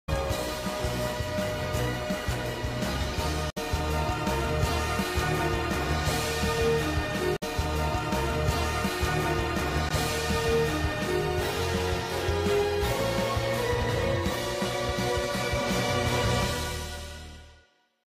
!!Clambonia!!\Transformation
!!This is Stacy the holder of the Clam Miraculous!!